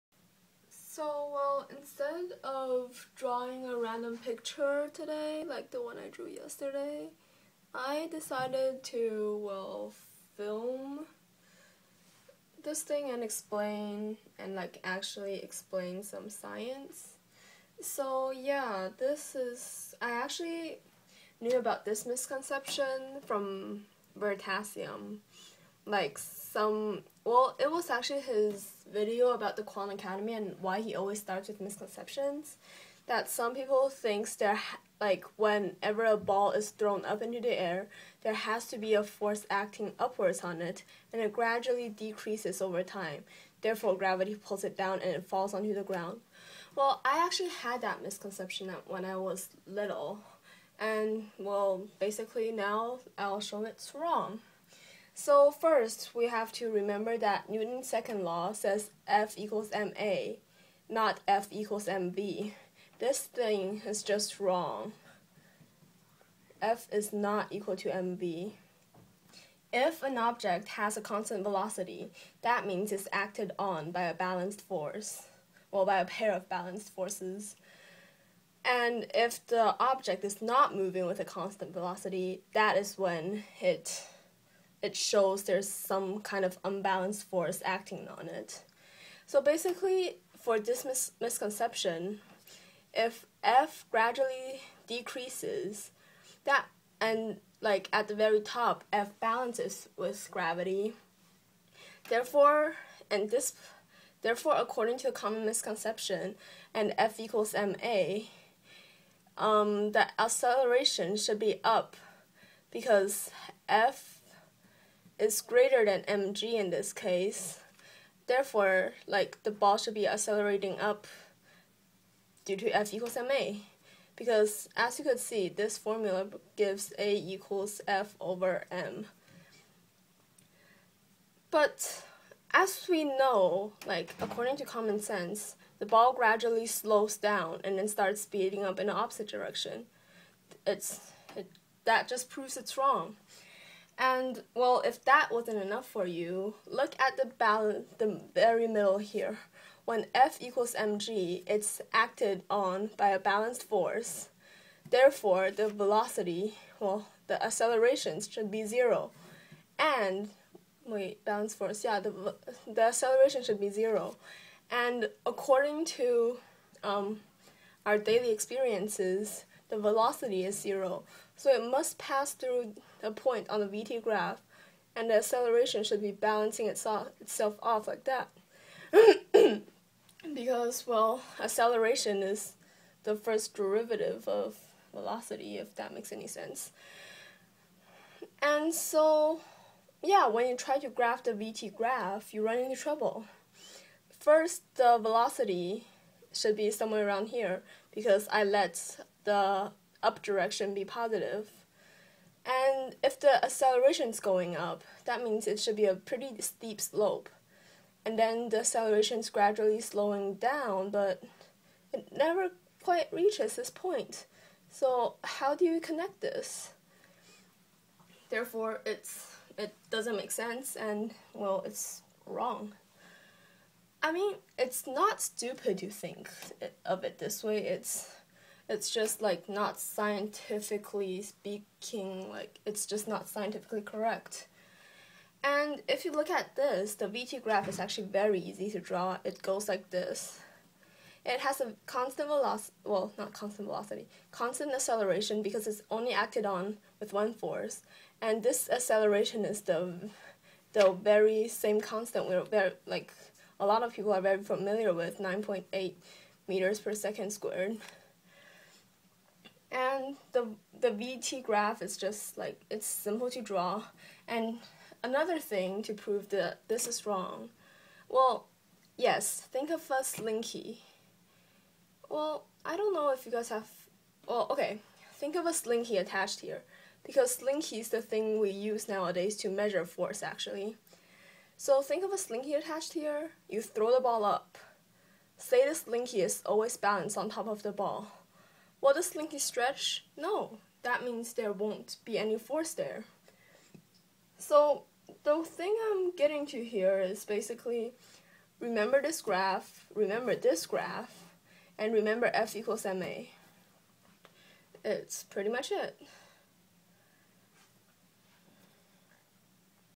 There's only gravity on the ball!
I know... I don't explain things well... This is pretty much the best I can do...

Recently I watched Veritasium's video about the Khan Academy again and realized there were posts below saying that there must be an upward force on objects when it's thrown up and in mid-air. Well, there might be another universe that works that way, but not here on earth.

To summarize: the ball is accelerating down during it's entire "flight". When it was going up, it was slowing down, therefore the ball was still accelerating down.

Newton's laws indicate that when an object as no forces acted on it, or when it has a pair of balanced forces, the object will move at constant velocity, therefore when the velocity changes, there must be a pair of unbalanced forces acting on the object. But if there is an upward force, the object should remain still at the middle point and should be accelerating up in the first half of the "flight".